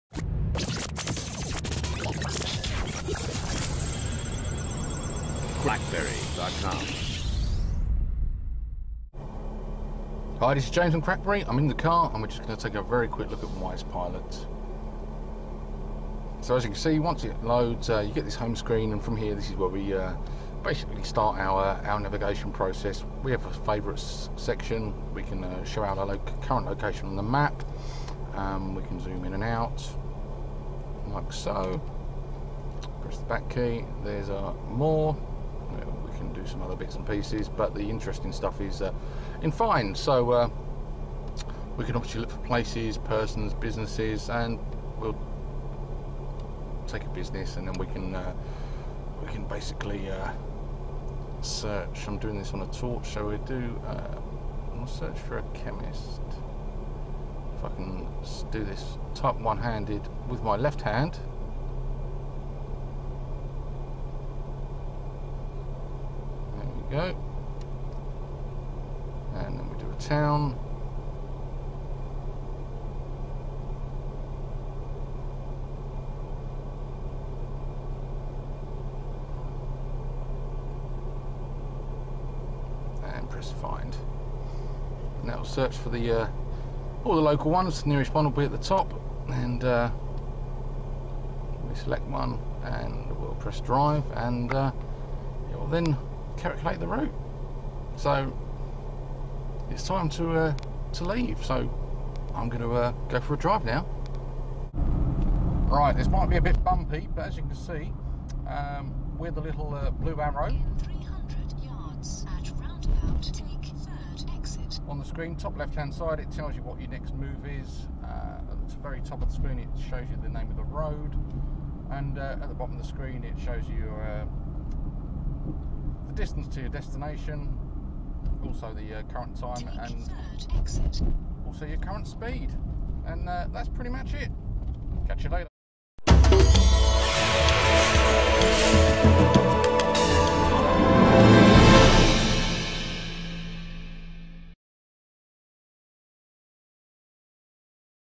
Wisepilot for BlackBerry Smartphones
Wisepilot is the third voice guided satellite navigation application I have reviewed here at CrackBerry in the last few months. Both Telmap and Gokivo were great apps and were free however there are a few things that set Wisepilot apart from those two. You can either continue using the free options or you can upgrade to use the premium serves. The question I have been asking myself is are they worth the money?